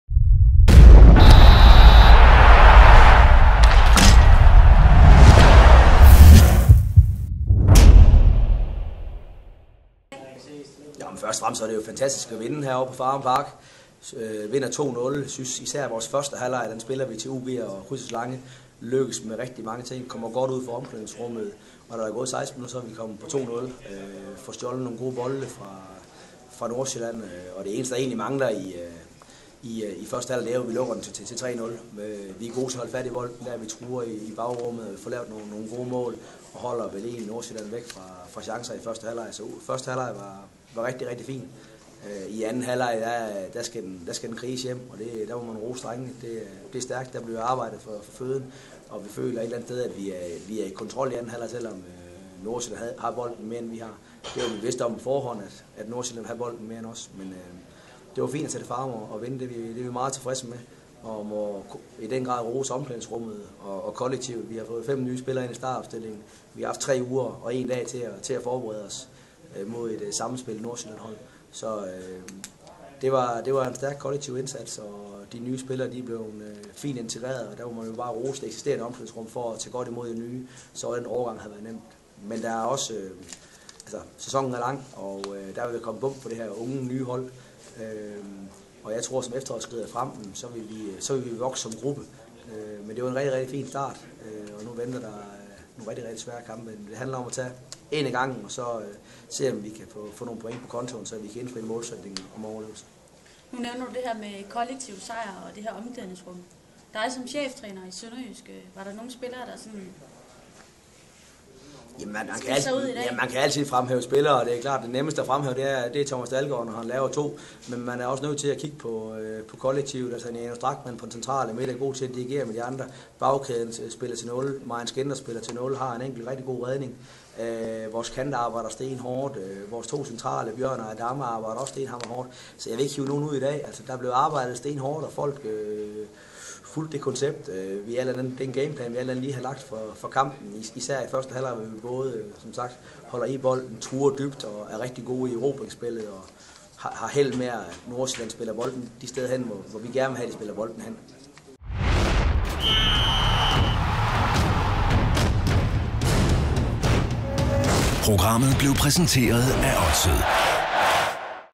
Michelsen: Kollektiv sejr
Jakob Michelsen og SønderjyskE åbnede Alka Superligaen med en 2-0-sejr hos FC Nordsjælland (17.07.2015).